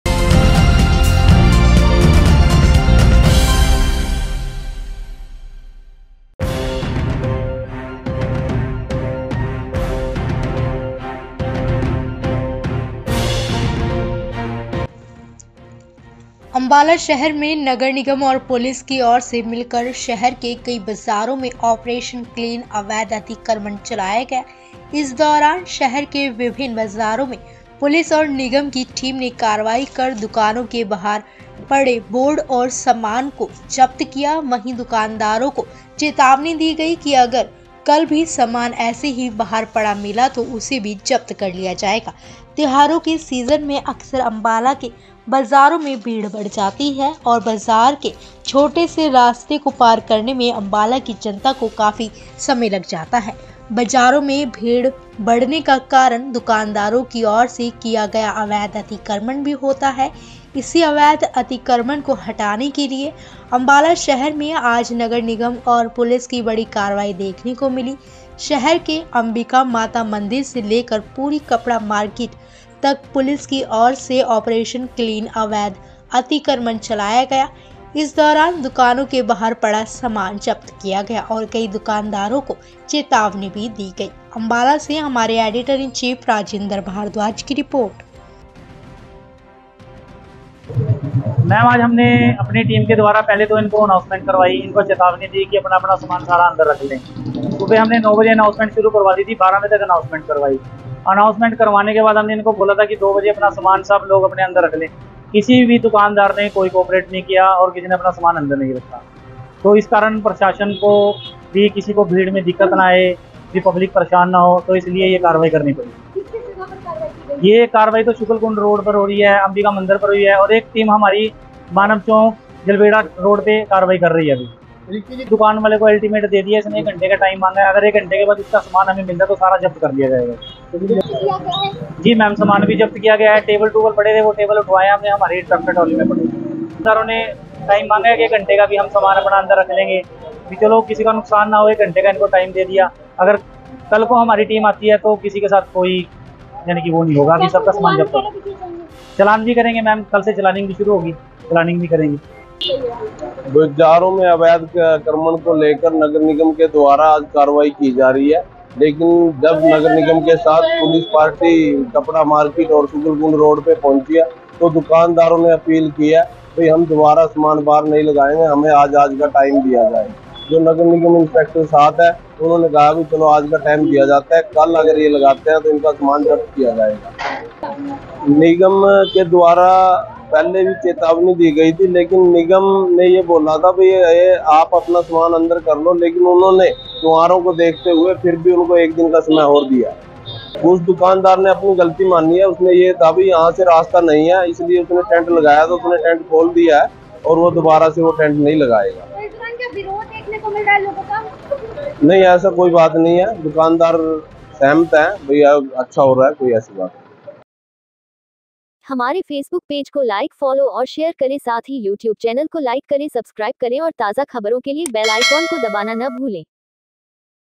नगर निगम और पुलिस की टीम ने चलाया ऑपरेशन क्लीन अवैध अतिक्रमणराजेन्द्र भारद्वाज। अम्बाला
नगर निगम और पुलिस की टीम ने चलाया ऑपरेशन क्लीन अवैध अतिक्रमण
राजेन्द्र भारद्वाज। अम्बाला